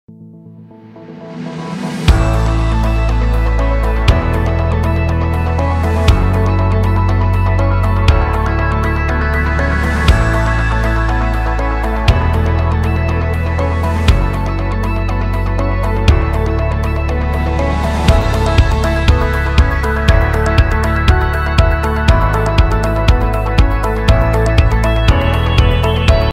Imagini cu apartamentul în care Delia locuiește cu Răzvan Munteanu, soțul ei |Libertatea
Află detalii: Imagini cu apartamentul în care Delia locuiește cu Răzvan Munteanu, soțul ei. A costat 1,4 milioane de euro